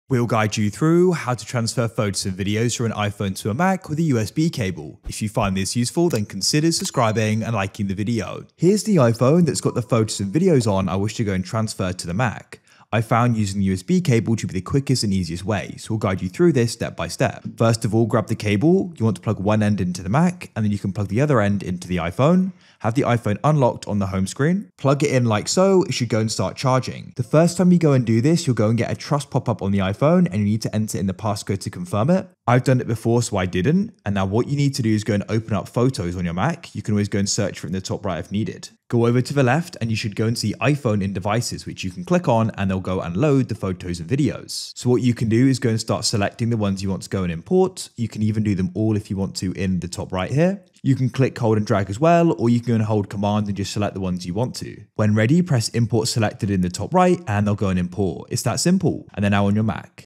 How To Transfer Photos & Videos From iPhone To Mac With USB Cable - Step By Step
Learn how to transfer photos and videos from iphone to mac with usb cable in this video.

GuideRealm